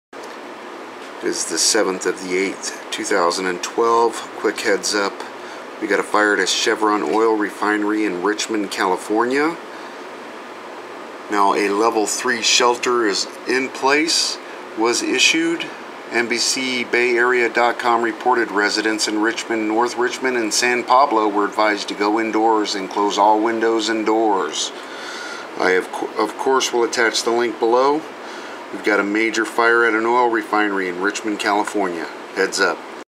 Richmond, California Alert (Fire at Chevron Oil Refinery) "level 3" Shelter in place!
Updated at 11:11 p.m. ET: A fire erupted at 6:15 p.m. Monday night at one of the processing units at the Chevron refinery in Richmond, Calif., the oil company said in a statement.

A "level 3" shelter in place order was issued, NBCBayArea.com reported. Residents in Richmond, North Richmond and San Pablo were advised to go indoors and close all windows and doors.